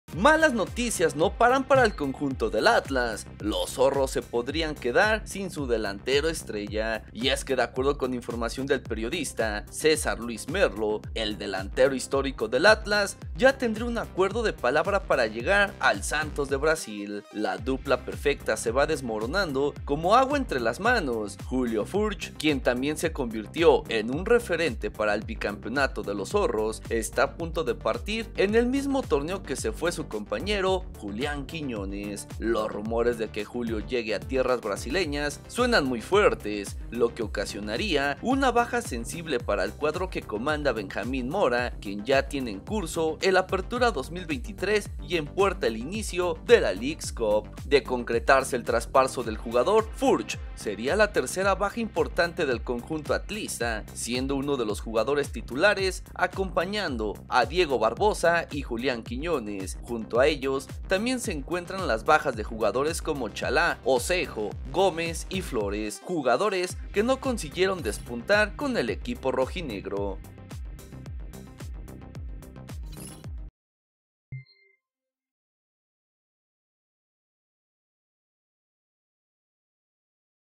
😱 ¡Se cae el Atlas! ⚫🔴 Julio Furch con un pie y medio fuera de Atlas ¿Se va al Santos de Brasil?
De concretarse el traspaso del jugador, Furch sería la tercera baja importante de Atlas